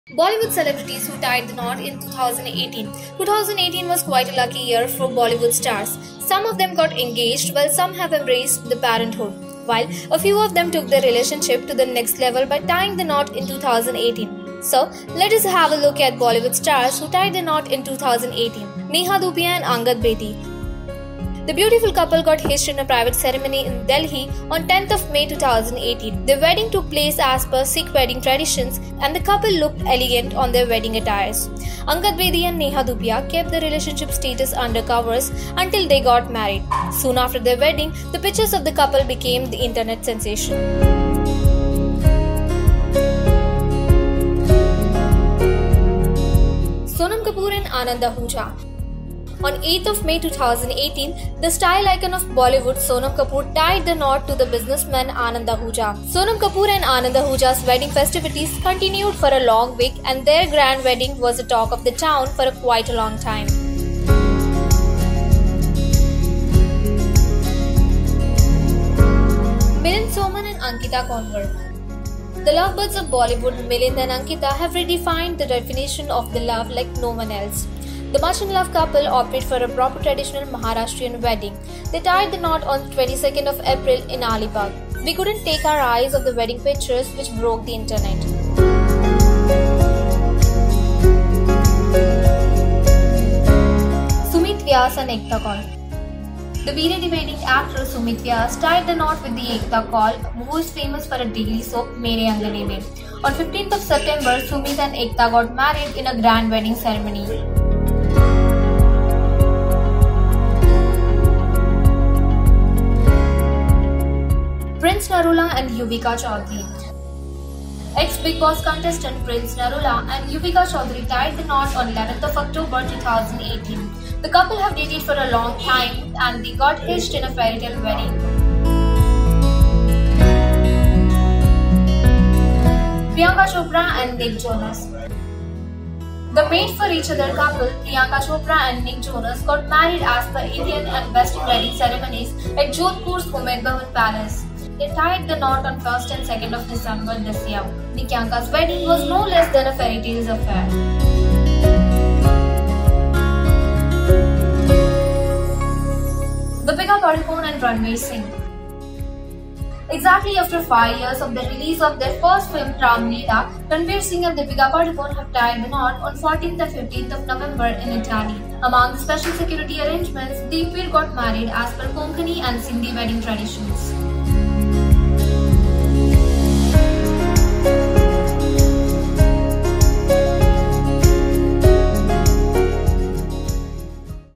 Bollywood Celebrities Who Tied The Knot In 2018
Here are the glimpse of Bollywood Celebrities Who Tied The Knot In 2018.